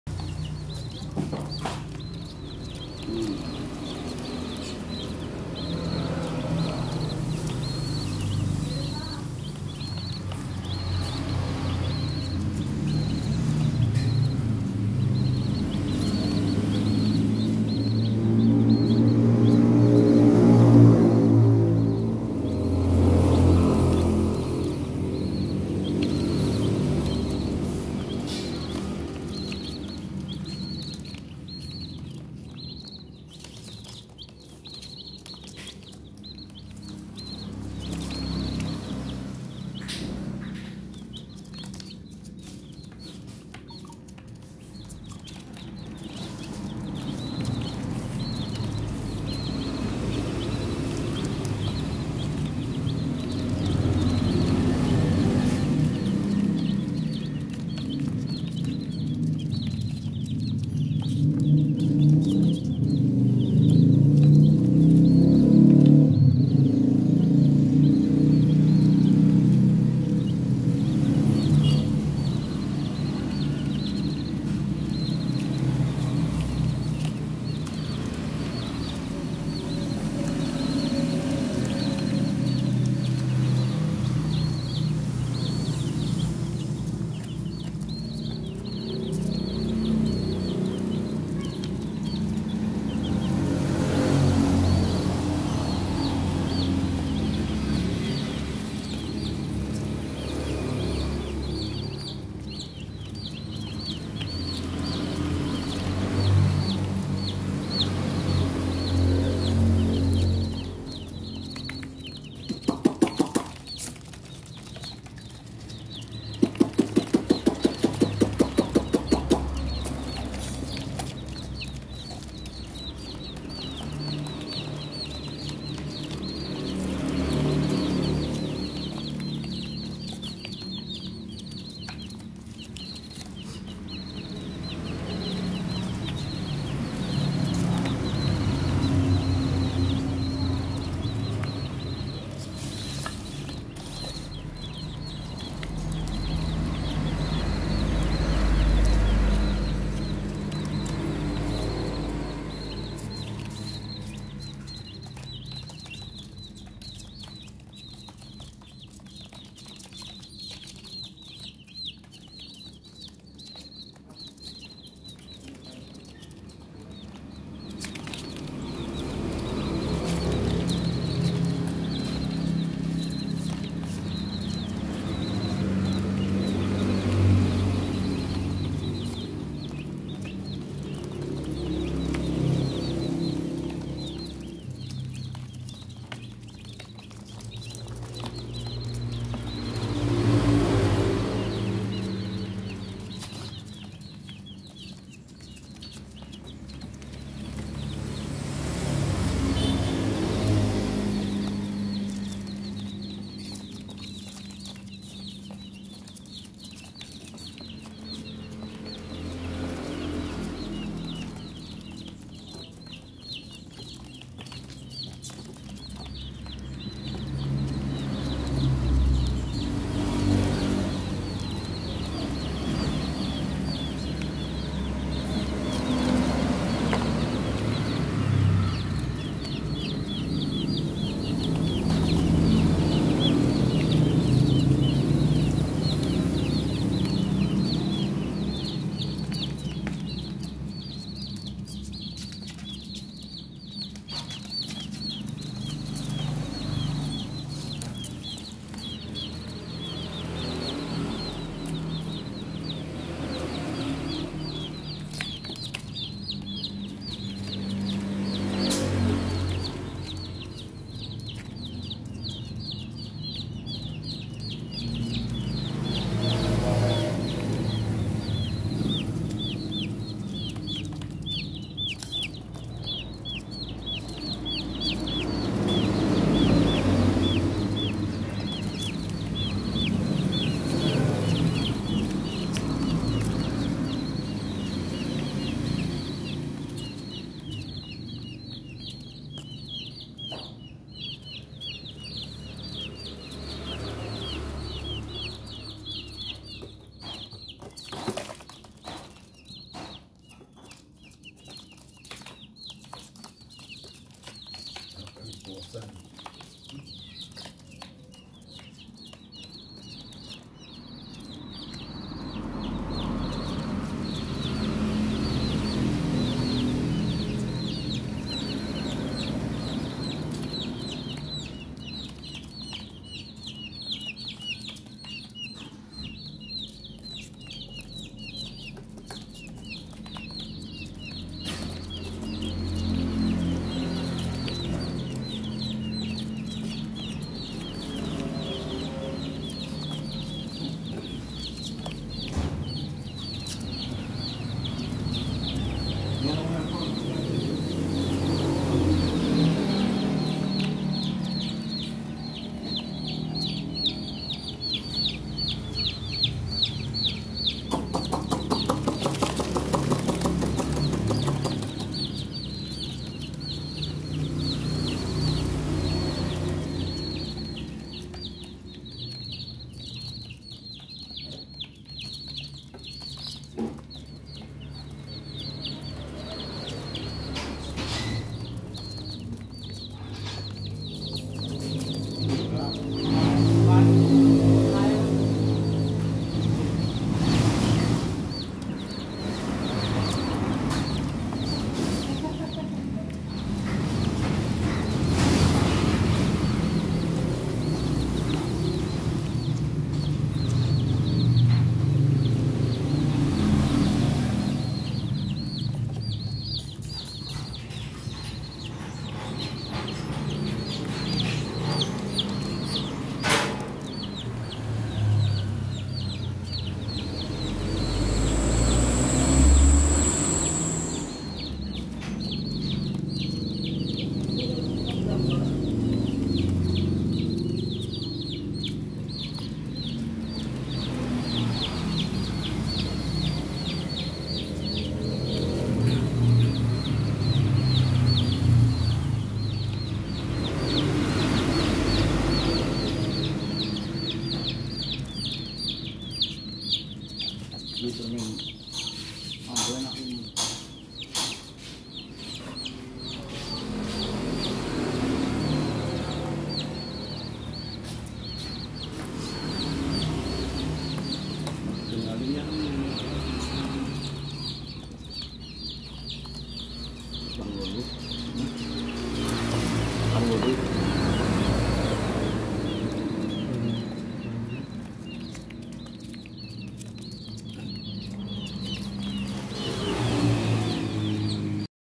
Chick Kicks The Ball While Eating Rice | Funny Chick | Schönes Küken
Das Küken tritt beim Essen gegen den Ball

the chick kicks the ball while eating